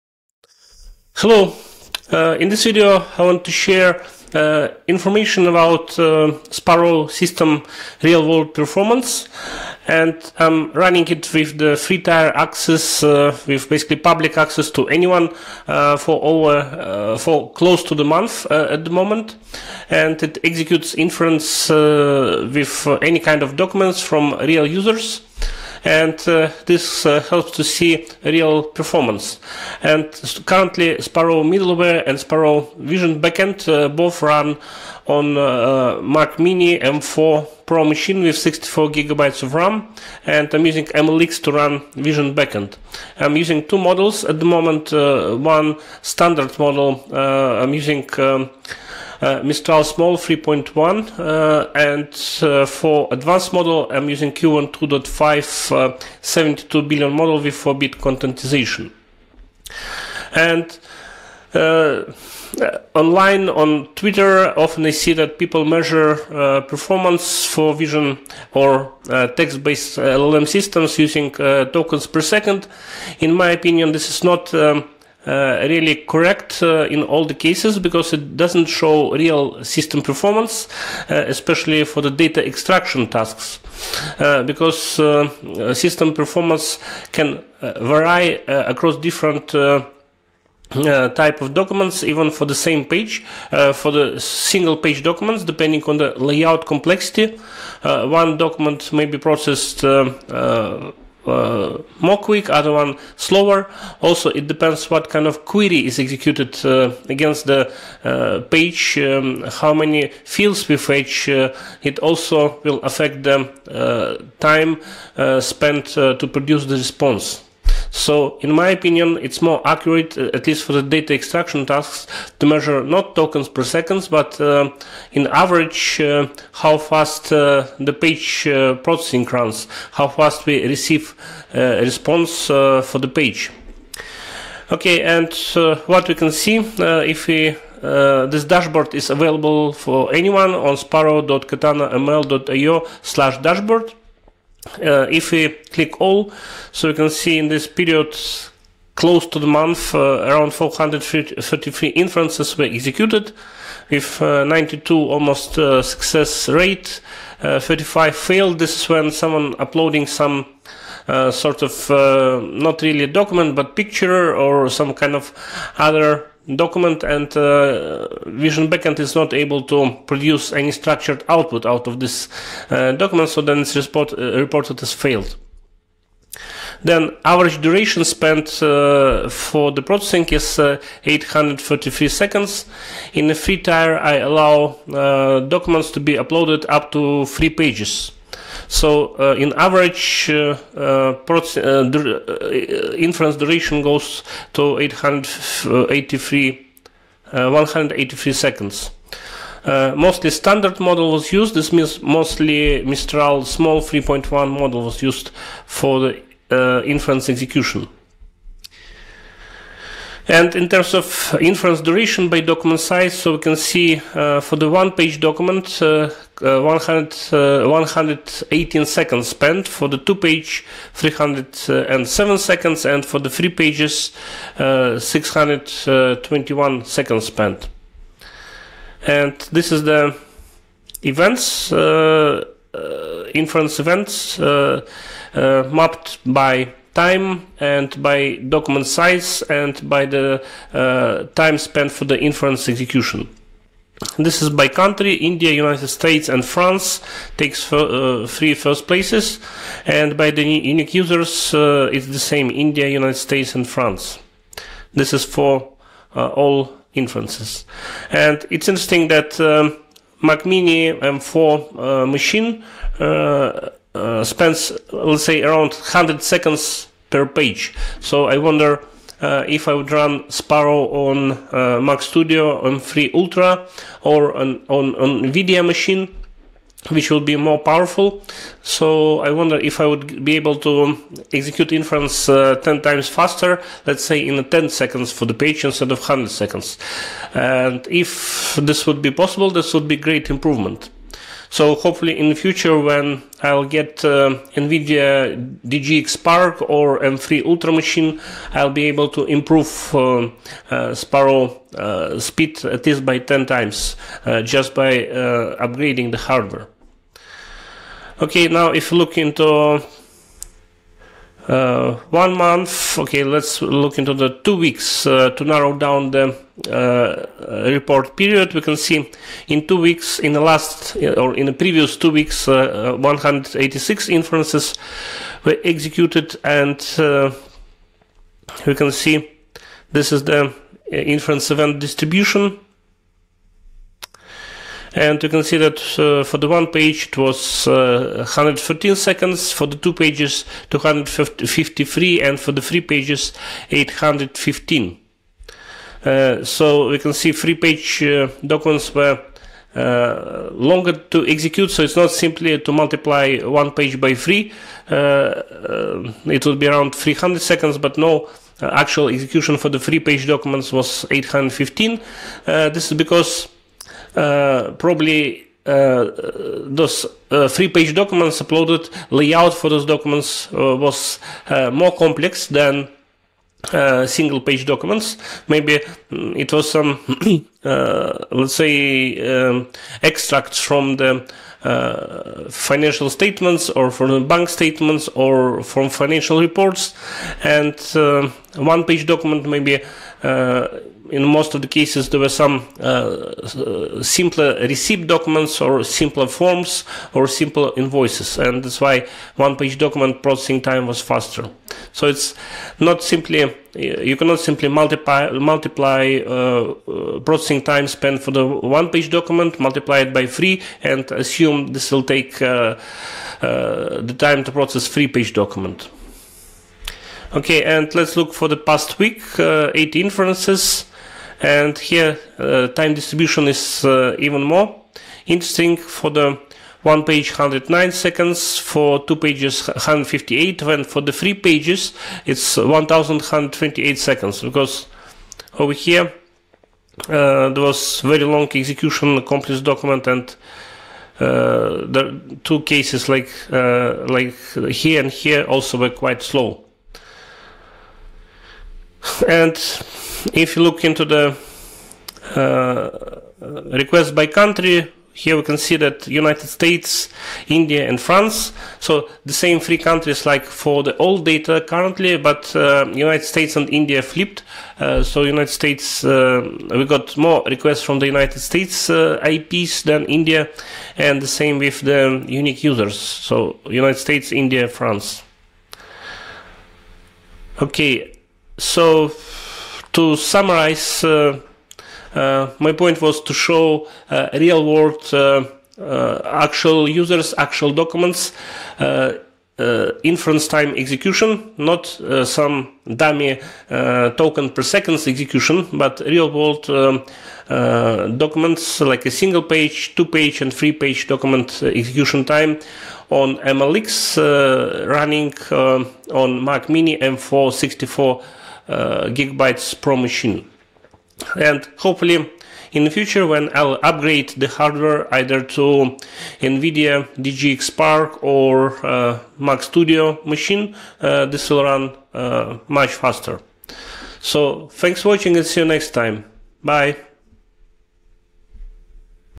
Vision LLM on Mac Mini M4 Pro: Real-World MLX Performance
I discuss the real-world MLX performance of Sparrow for structured data extraction with public access. The current Sparrow online instance runs on a Mac Mini M4 Pro with 64GB of memory. On average, it processes one page in 100 seconds. I explain why tokens-per-second measurements can be misleading when evaluating structured data extraction.

0:00 Intro
0:32 MLX for Vision Backend
2:21 Sparrow Dashboard
2:42 Stats Explained
8:00 Summary